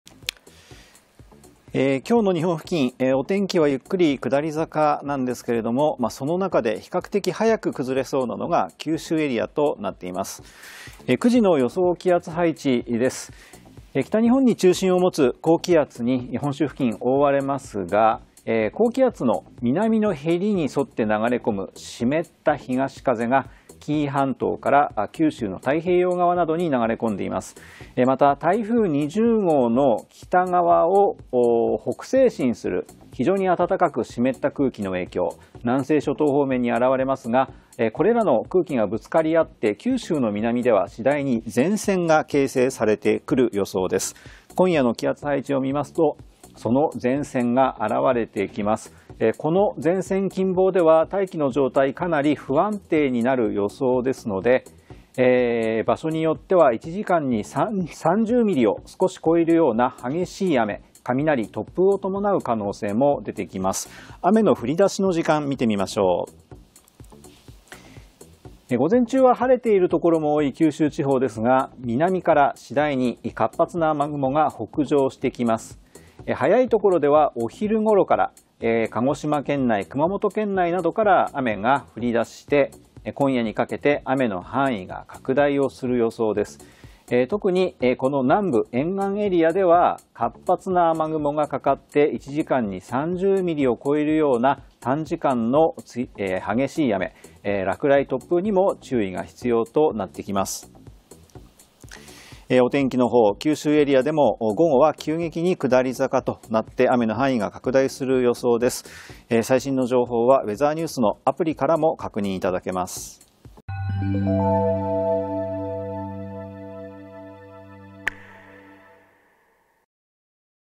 【前線が通過】九州は夕方から本降りの雨
午前中は日差しが届く九州は、午後になると雲が厚みを増してきます。段々と雨の降り出す所が多く、夜にかけて南部から本降りの雨になる見込みです。

お出かけのタイミングで晴れていても、帰りに備えて雨具をご用意ください。